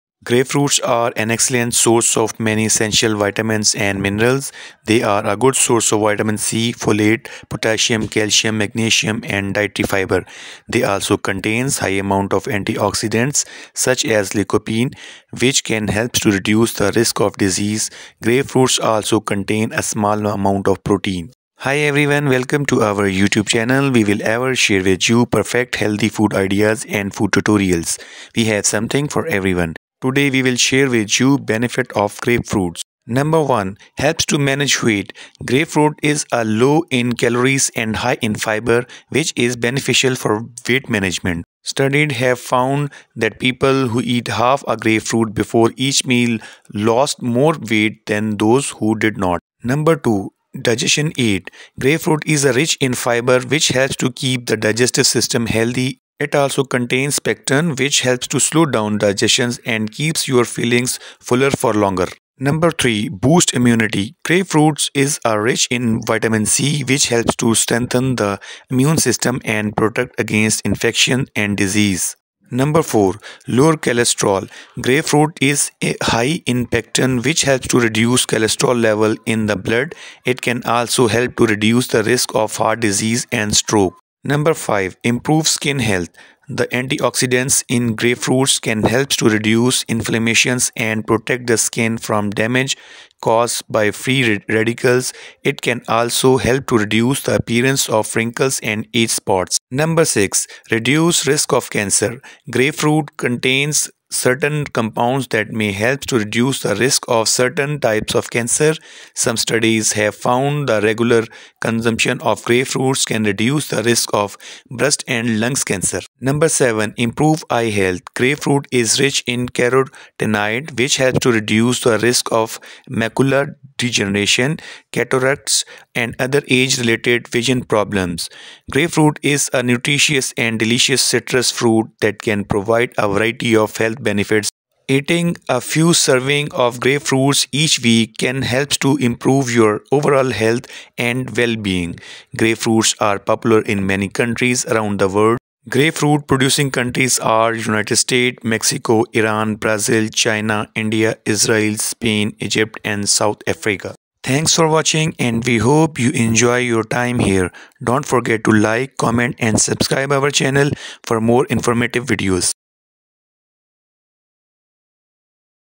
Revealing the Unbelievable Health Benefits of Eating Grapefruit
Grapefruits are an excellent source of many essential vitamins and minerals. They are a good source of vitamin C, folate, potassium, calcium, magnesium, and dietary fiber. They also contain high amounts of antioxidants, such as lycopene, which can help reduce the risk of disease. Grapefruits also contain a small amount of protein.
Here are some of the top health benefits of eating Grapefruits:
1. Help Manage Weight
2. Aid Digestion
3. Boost Immunity
4. Lower Cholesterol
5. Improve Skin Health
6. Reduce the Risk of Cancer
7. Improve Eye Health